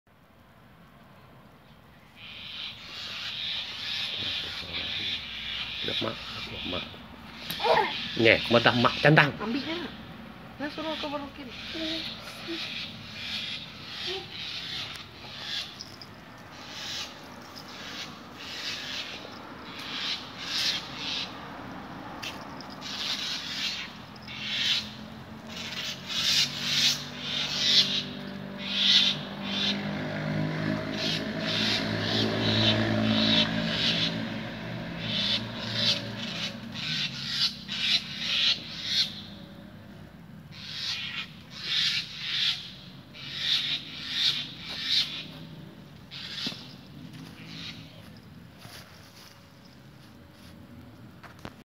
Inilah jenis burung disarawak borne... kalau iban panggil namanya ialah burung bunuh.
Burung bubut mencari makan